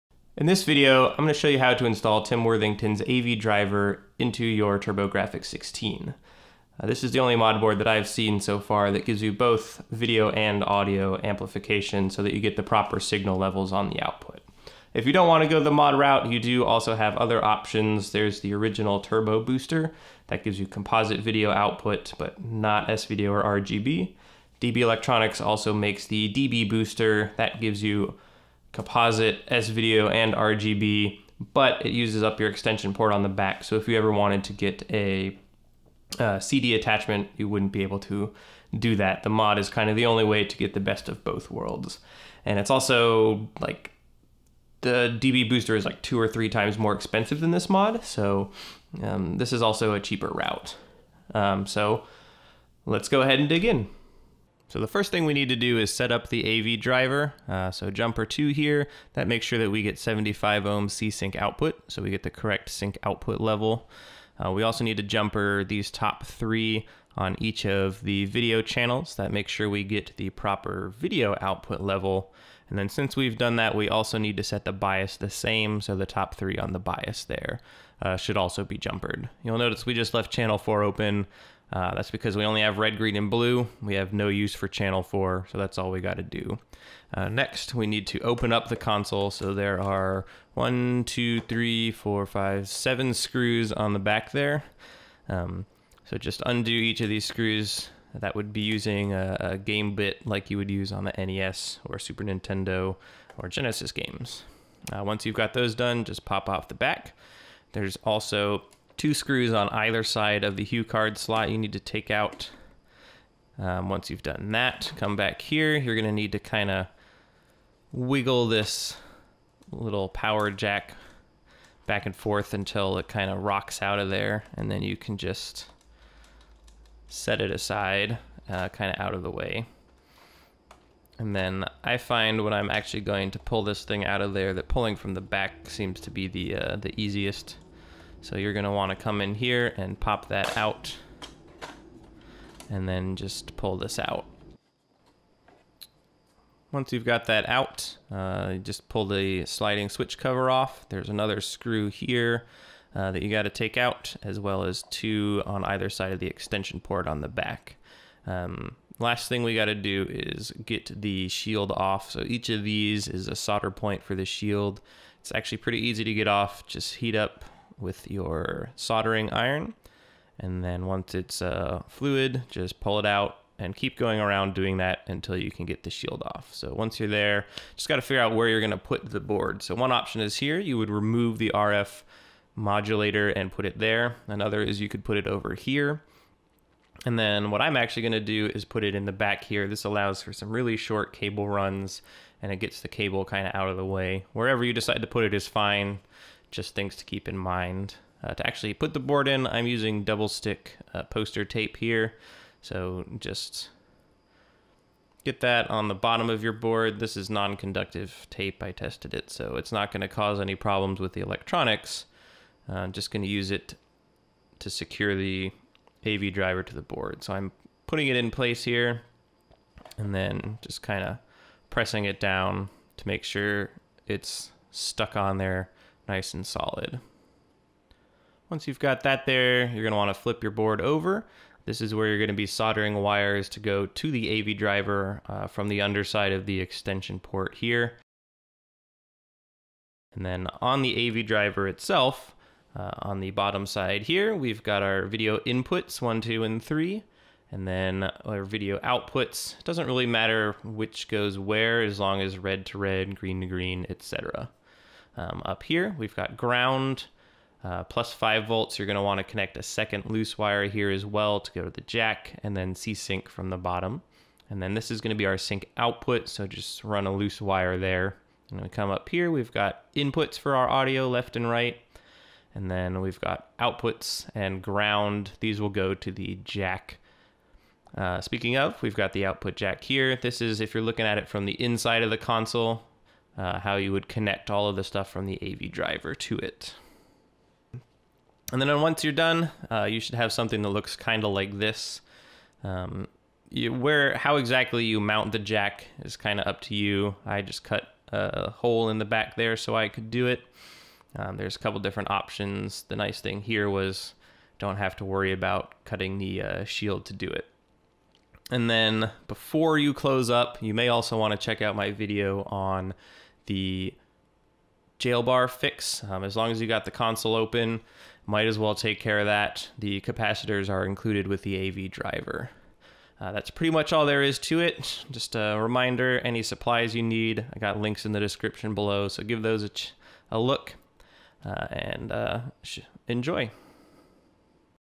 How to get RGB from your TurboGrafx 16 using the AV Driver
In this video, I show you how to install Tim Worthington's AV Driver in a US TurboGrafx 16 so that you get properly amplified video and audio output. You will need:

A SCART cable with no resistors/capacitors in it (I recommend upgrading to the coaxial version)

And a few tools, namely a soldering iron and solder, some wire, phillips screwdriver, game bit, and something to put holes in your console with (drill, saw, whatever).